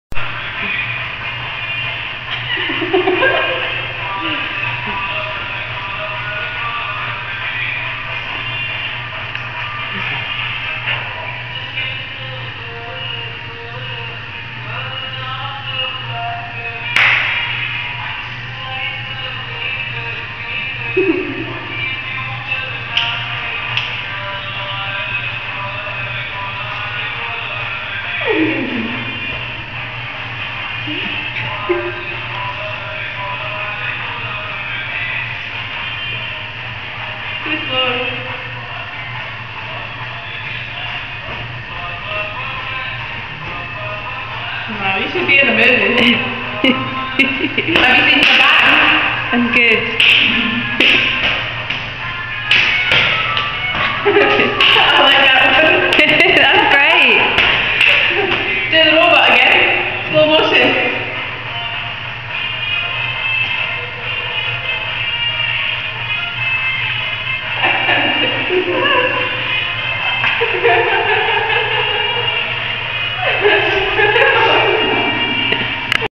Bollywood Das
This is dancing Das. We stayed with him and his wife in a home-stay in beautiful Varkala. He was a very special dancer!! A bit like the Indian version of Eddie Murphy!